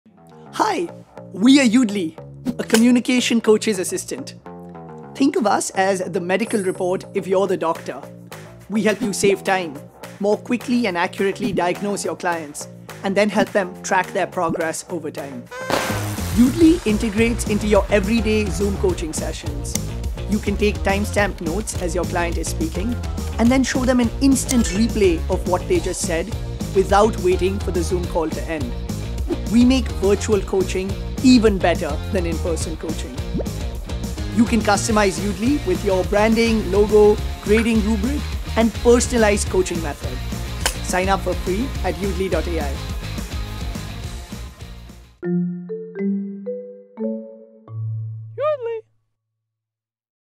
Yoodli for communication coaches: Speech coaching assistant
Learn how thousands of communication coaches are saving time and driving better client outcomes If you're a media trainer, sales coach, speech coach, or communicators professor, this video is for you! Get an immediate transcript of your client's speech, show them an instant replay during virtual speech coaching, and wow your clients with AI powered objective data on their eye contact, pacing, and more!